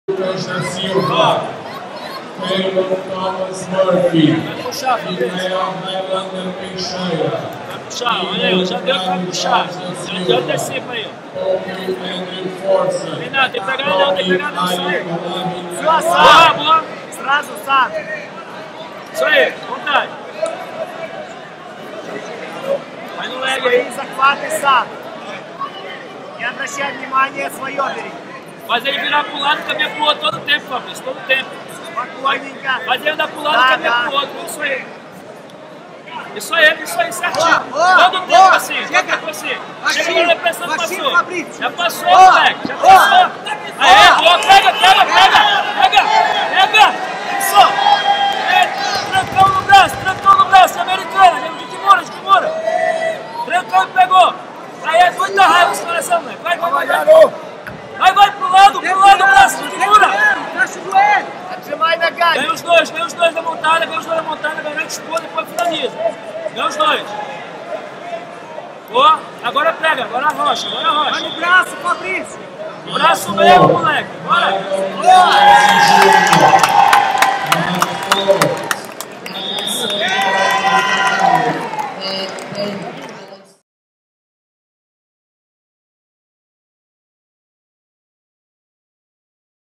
Fabricio Andrey VS Yaroslav Blazhko / European Championship 2020
Brown-Belt/Adult/Male/Feather - Semifinal